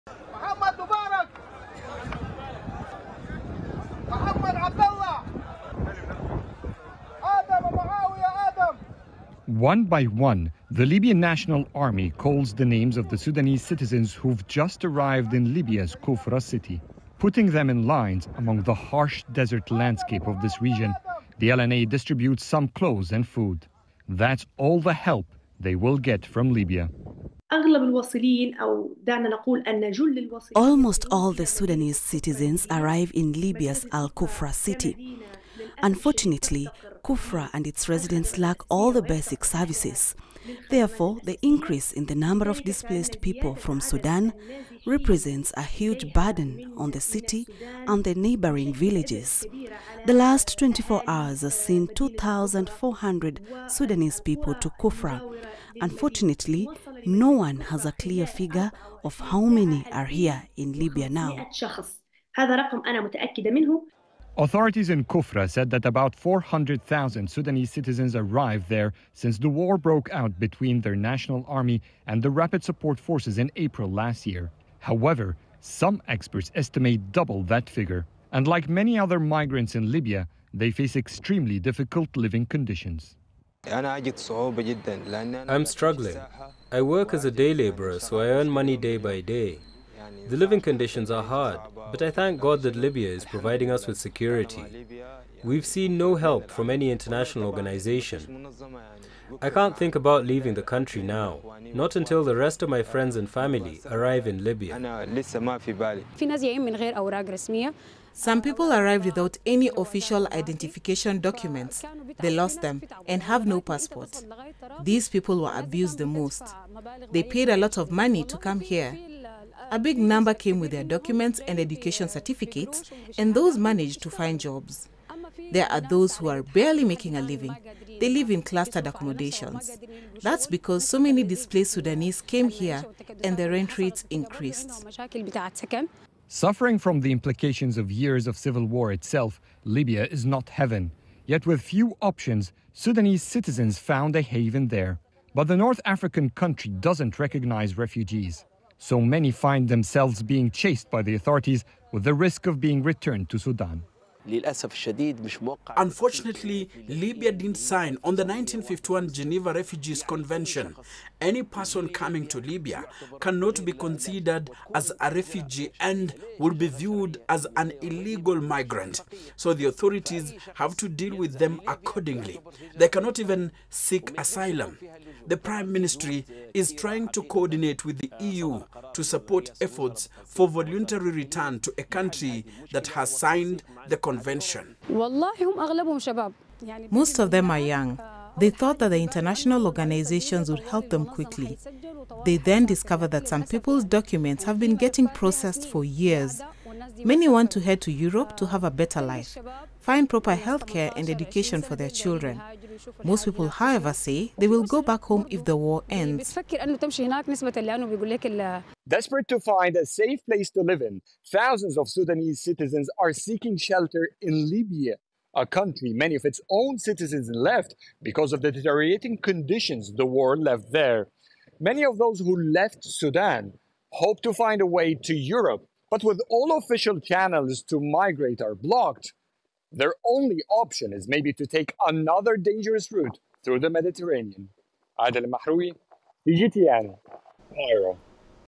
Fears rise over fate of Sudanese refugees in Libya
Sudanese citizens found a haven in Libya despite the North African country also grappling with a long-running civil war. However, Libya does not recognize refugees and so many people find themselves being chased by the authorities, with the risk of being returned to Sudan.